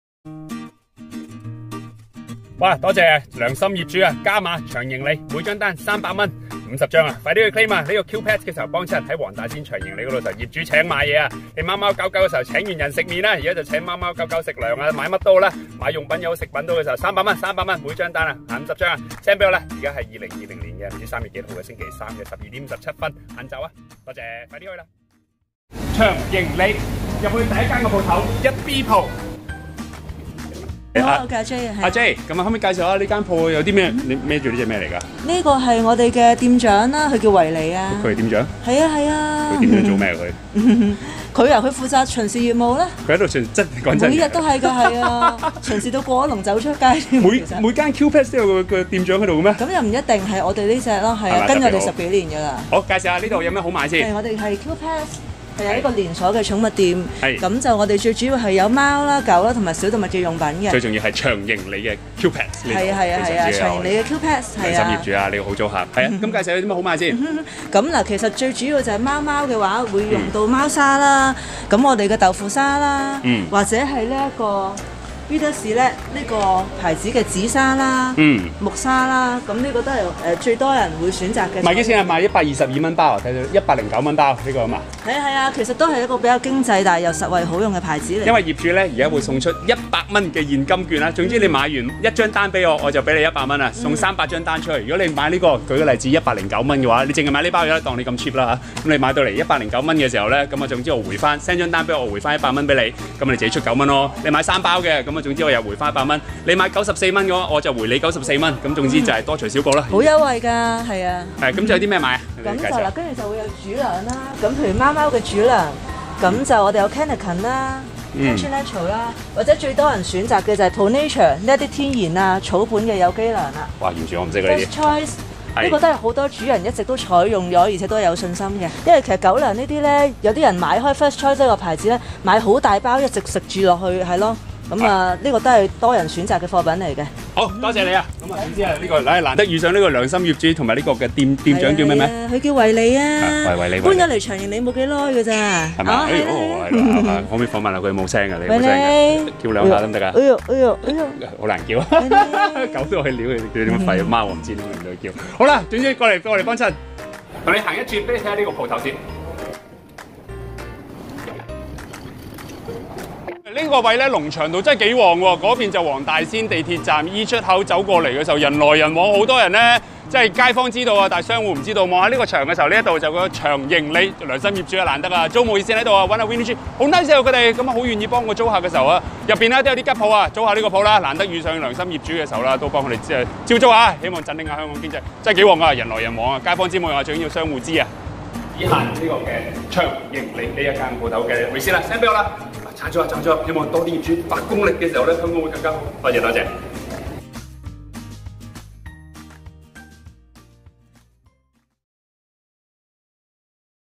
多謝良心業主(翔盈里)Q-Pets加碼 - 每張單$300 x 50張。 今個月(3月份)翔盈里業主請大家隻貓狗到 Q-Pet 食嘢/玩嘢/用嘢 (每張單 claim 限300元)。
多謝良心業主(翔盈里)Q-Pets加碼 每張單$300 x 50張。

今個月(3月份)翔盈里業主請大家隻貓狗到 Q-Pet 食嘢/玩嘢/用嘢 (每張單 claim 限300元)。

今個月(三月份)， 黃大仙翔盈里業主請大家隻貓貓狗狗到 Q-Pets 食嘢/玩嘢/買嘢 (每張單補貼300元)。

幫襯翔盈里租客 Q-Pets (黃大仙龍翔道110號翔盈里三樓1B舖，入口盈鳳里，近鳳德公園)，業主三月份送你現金300元，限50張單。

在店內購買 Q-Pets 任何商品 ，頭300元業主會全數支付。
例如: (1) 張單合共是$90，業主補貼$90。
(2) 張單價合共是$390，業主補貼$300。
(3) 張單價合共是$1900，業主補貼都是$300。
按整張單據計算，限50張單。必須是翔盈里的分店收據。

每個電話號碼只可提供一張單據， 避免濫用... u know!

買完，Whatsapp返張收據比我(李根興 Edwin) (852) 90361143，並註明收款方式 Payme / Alipay / FPS (最好有QR Code) 或銀行戶口轉數 (必須提供戶口名稱的英文全名)。我們會幫業主盡量數日內會轉數給你!

三月份內限送50張單(claim up to $300 each)，名額爆滿會馬上在Facebook《李根興創業之友》通知。

難得良心業主，位置人流是好靚，街坊個個知，可惜唔夠商戶知。翔盈里租務熱線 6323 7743  (聯絡 Winnie Chu) - 業主超 nice!

租客《Q-Pets》 全港最大店網寵物連鎖店
地址: 黃大仙龍翔道11號翔盈里1B舖 (入口盈鳳里，近鳳德公園，黃大仙站 MTR Exit E)
營業時間: 中午12至晚上十時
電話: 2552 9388